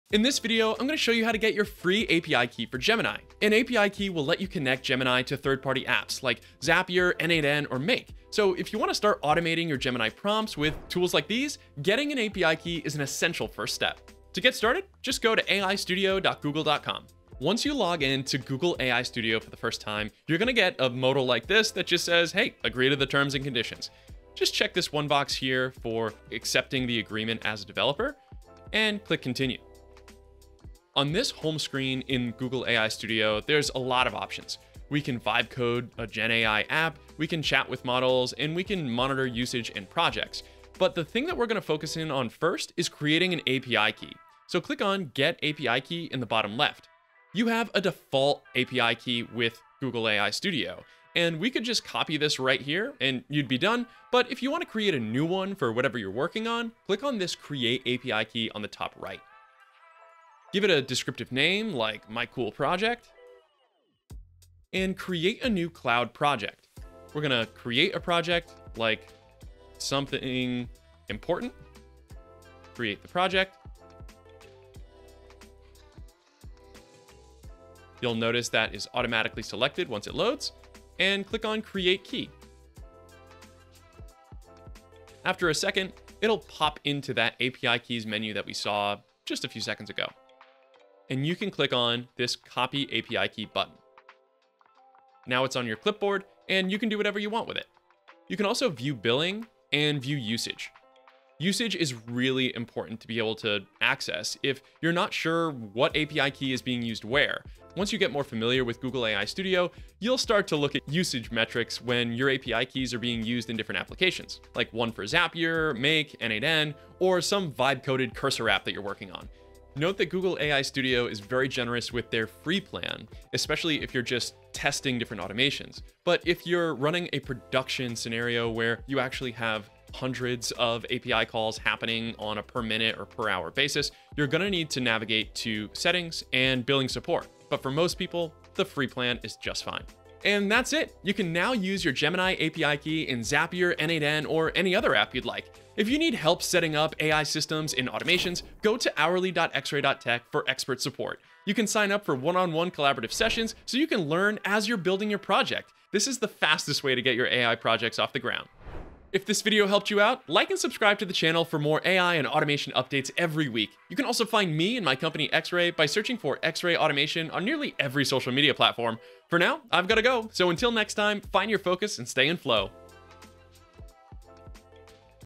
How to Get Your Gemini API Key in 2026
Learn how to get a Gemini API key so you can connect Gemini to automation tools like Zapier, n8n, Make, and other third-party apps.
Need help with an AI project?
00:00 - Intro
00:14 - Get your Gemini API Key
01:38 - Usage and billing
02:20 - Get AI support from XRay